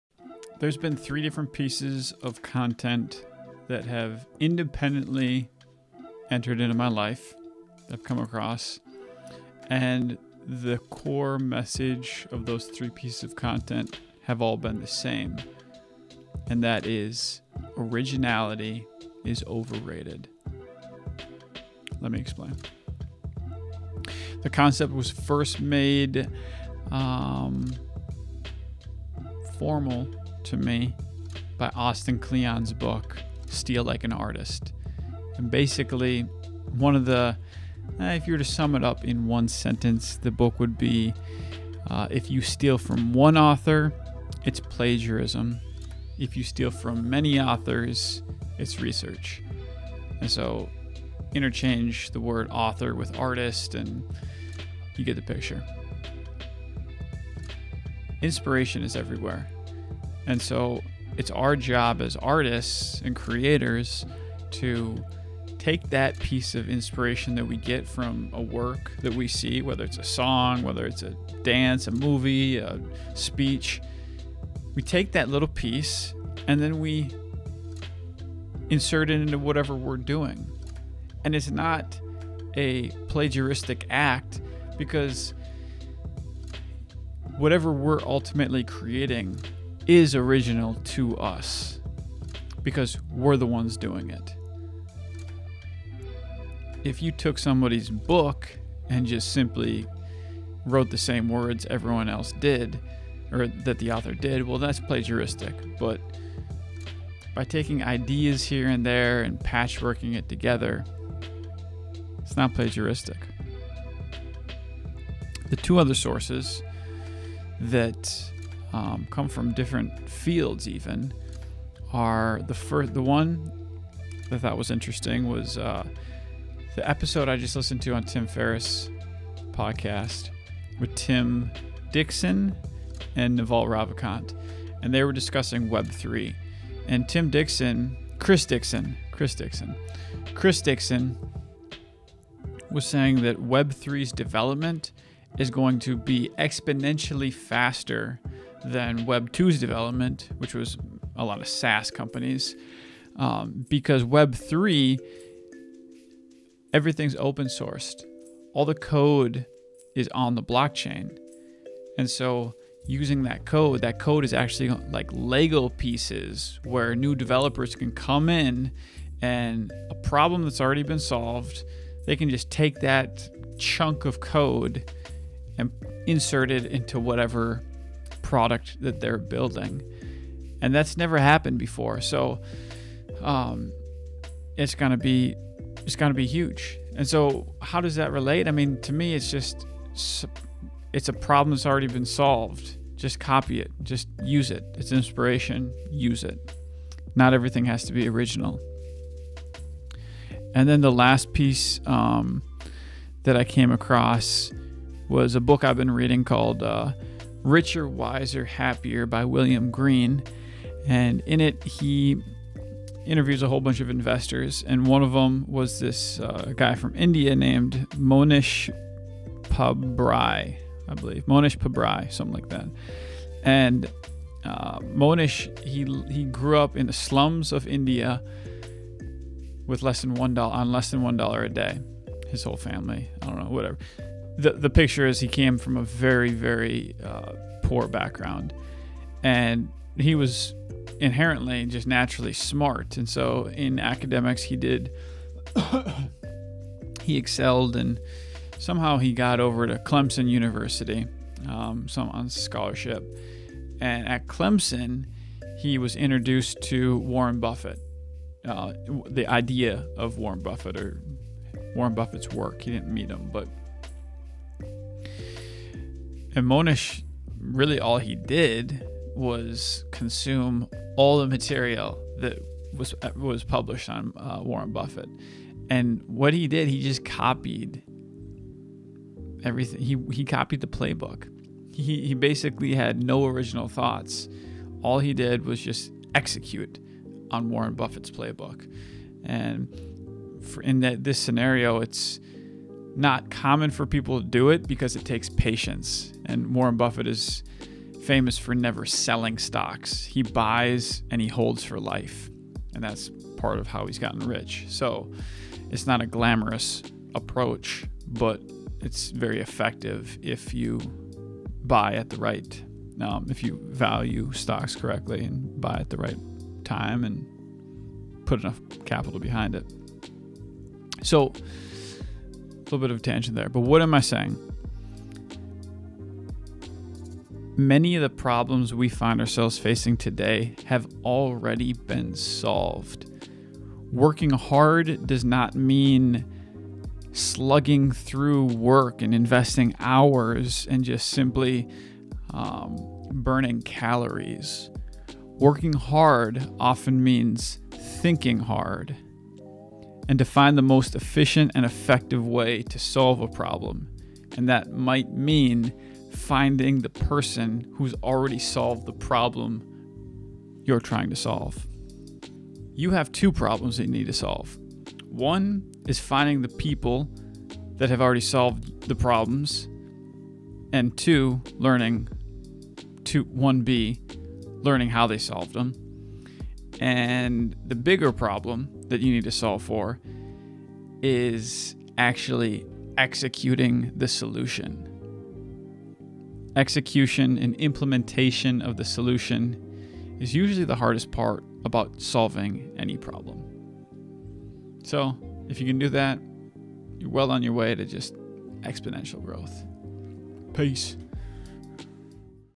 Originality is Overrated
3 separate pieces of content came across my desk that all pointed towards the same thing, originality is overrated.